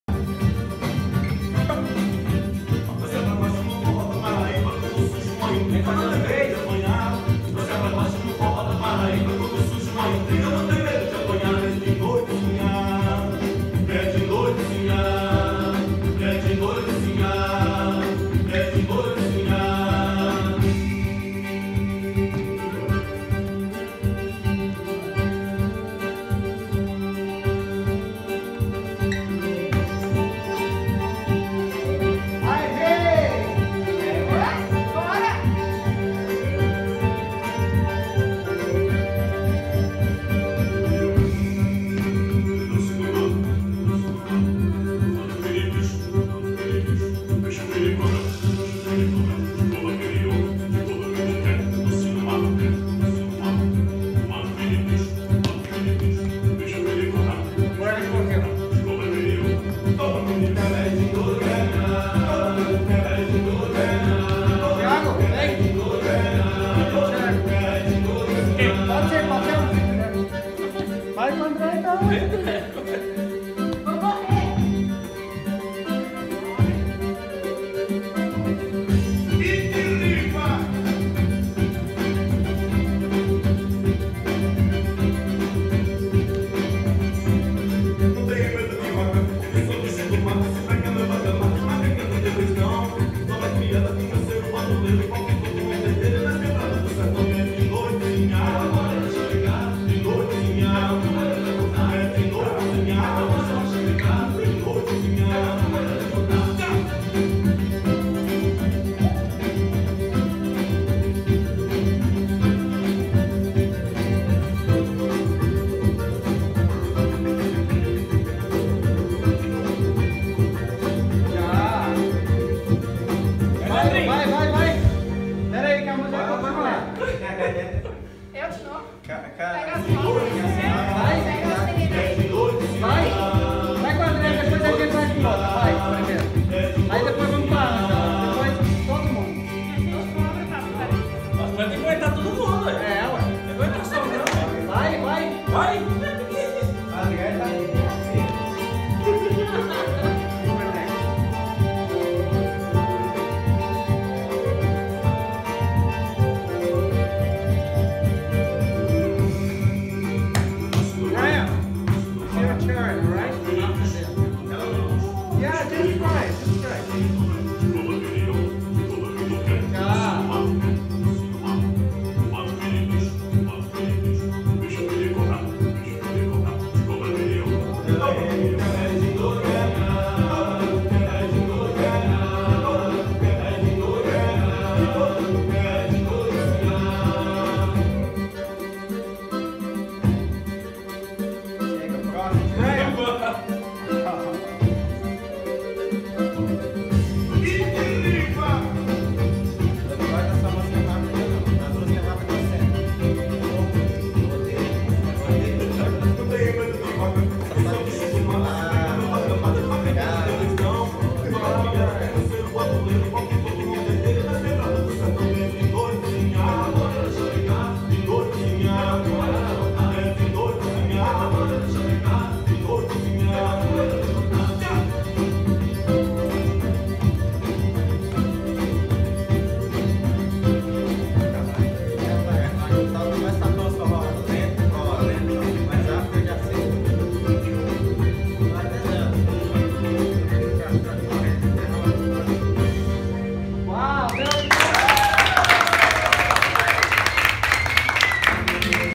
Quando me fizeram praticar o passinho com todos os meninos da aula. April 28, 2019
Nós somos "Os Forrozeiros" um grupo que promove o Forró em Boston, Estados Unidos.
Nos siga no Instagram e facebook: @Forrofestusa